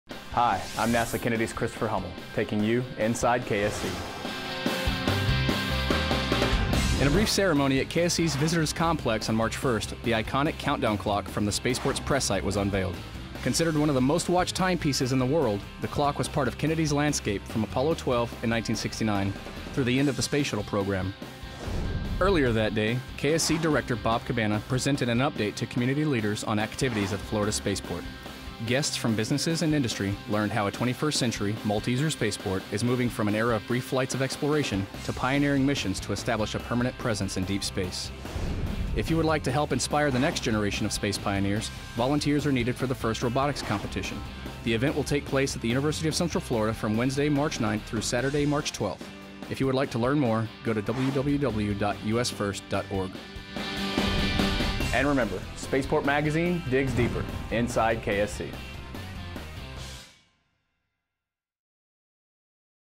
Inside KSC! for March 11, 2016
Kennedy Space Center's iconic countdown clock finds a new home, the spaceport's director briefs community leaders and volunteers are needed for the upcoming FIRST Robotics Competition -- all are topics covered in this week's edition of Inside KSC!